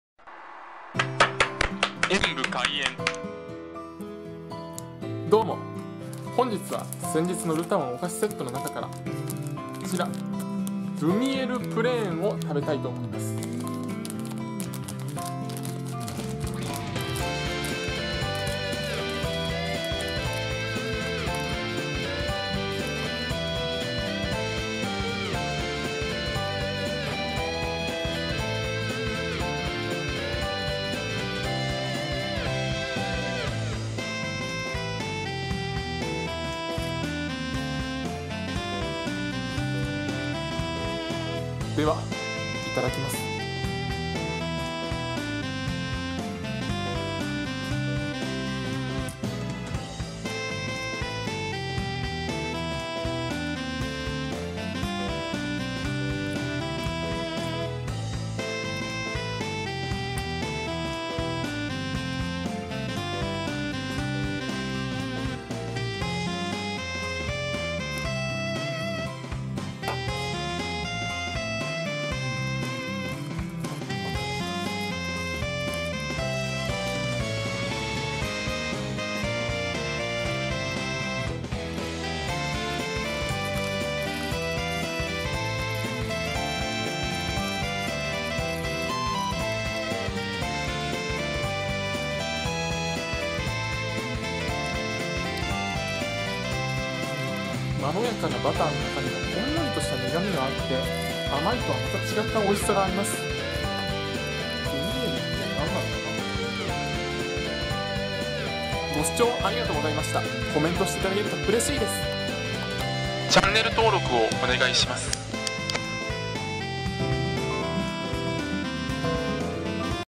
ルタオ ドゥミエルプレーンを食べてみた
ルタオ　ドゥミエルプレーンを食べてみました。
さっくりシンプルな中にも仄かな甘みがあって～